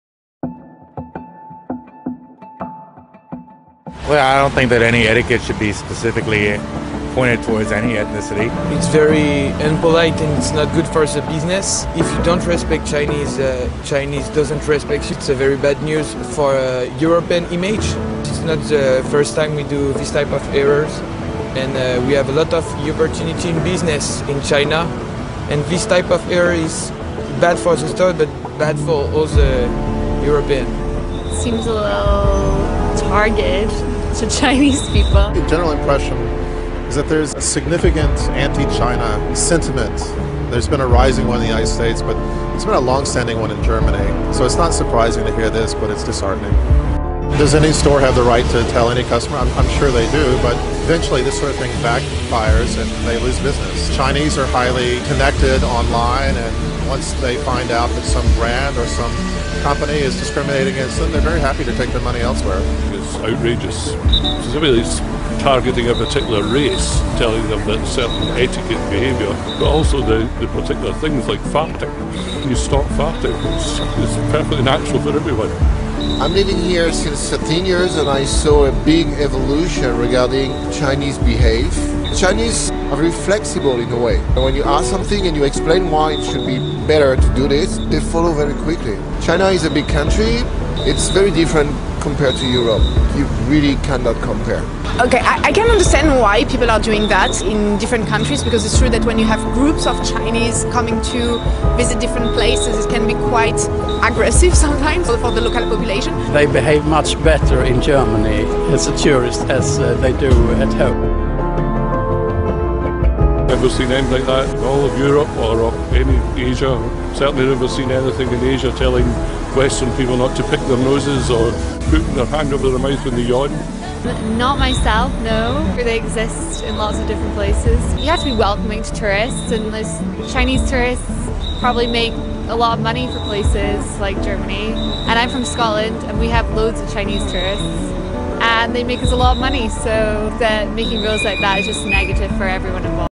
外国人怎么看待歧视
你有见过针对中国人以及不尊重中国人的语言吗？在外国人眼里，不尊重中国人的后果有哪些？